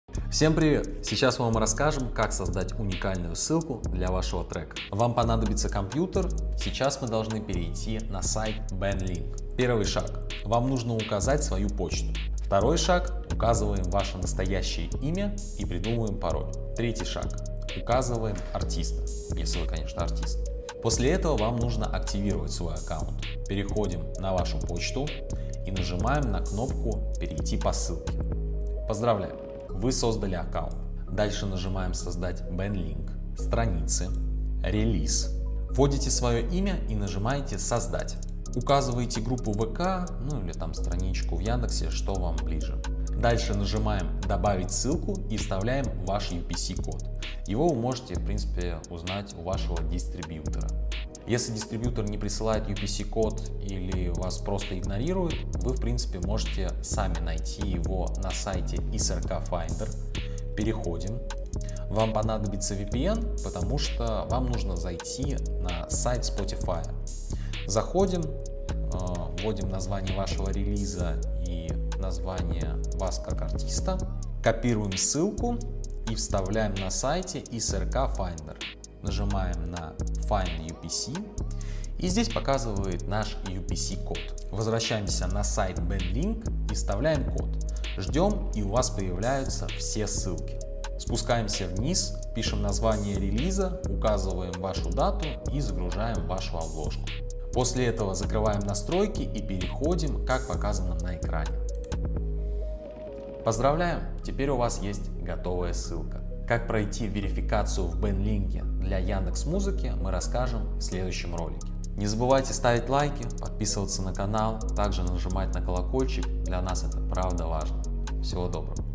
Создание УНИКАЛЬНОЙ ССЫЛКИ для трека: Пошаговая инструкция / Регистрация в BANDLINK
0:00 — Приветствие
0:10 — Создание аккаунта
0:51 — UPC код. Где взять?
1:24 — Создание уникальной ссылки
1:46 — Заключение